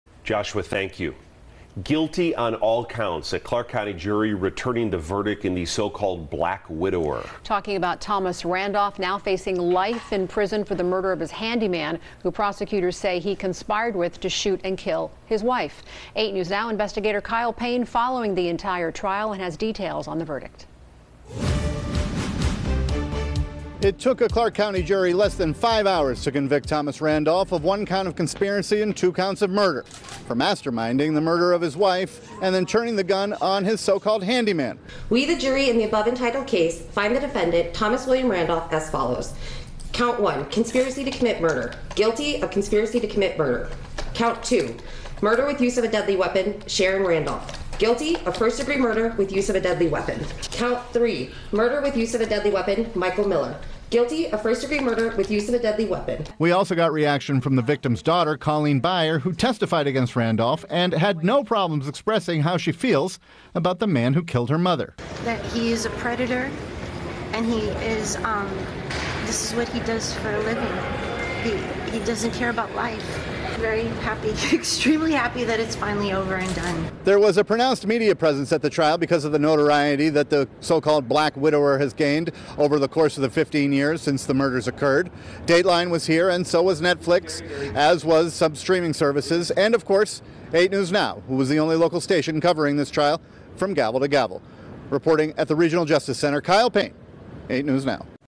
So-called ‘Black Widower,’ Thomas Randolph verdict announced
A verdict was announced in Thomas Randolph’s retrial for the deaths of his wife and purported handyman Thursday afternoon.